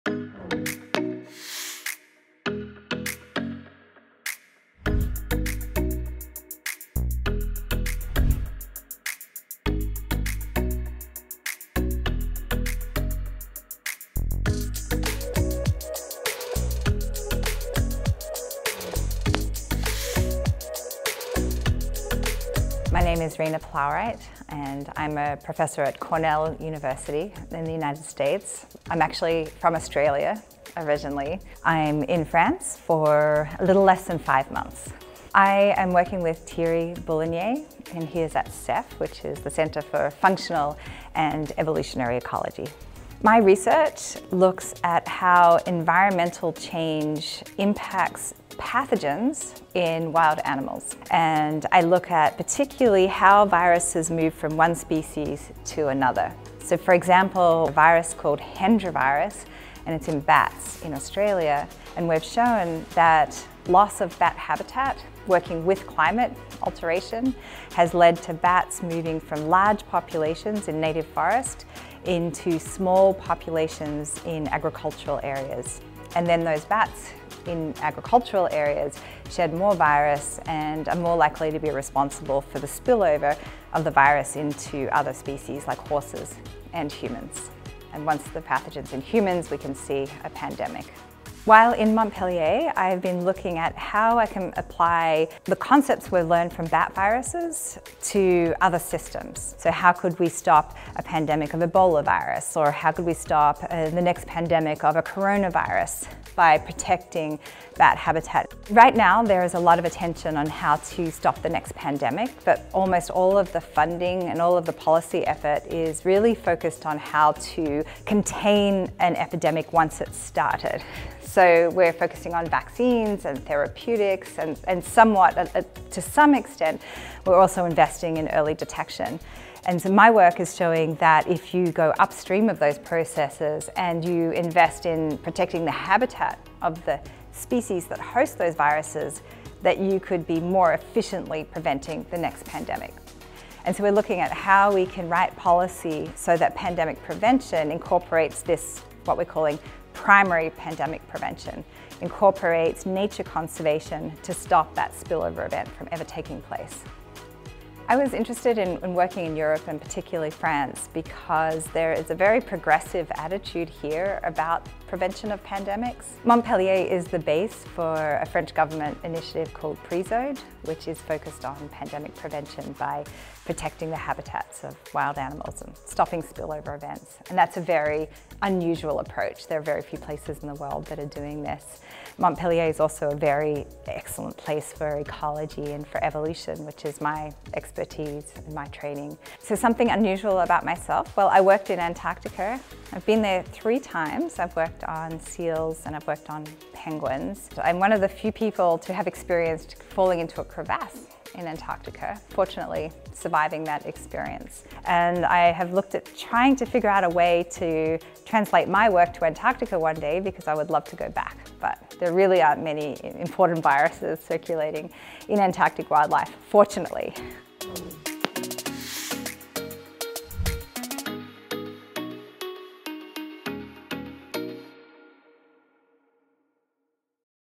PORTRAIT / MAK'IT accueille RAINA PLOWRIGHT
RAINA PLOWRIGHT
▪ PROFESSEURE EN ECOLOGIE DES MALADIES
▪ UNIVERSITE DE PURDUE (ETATS-UNIS)
▪ Dates de séjour : 01/09/2022 – 19/01/2023

▪ PROJET :
STRATÉGIES PROACTIVES DE PRÉVENTION DES PANDÉMIES